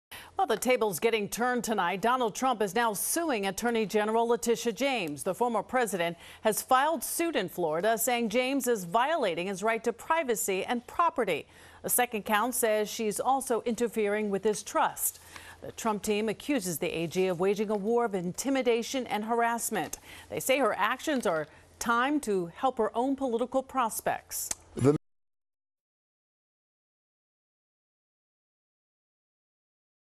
Former President Trump announces he is suing NY AG Letitia James
Former President Donald Trump announced on his Truth Social media platform that he is suing New York Attorney General Letitia James.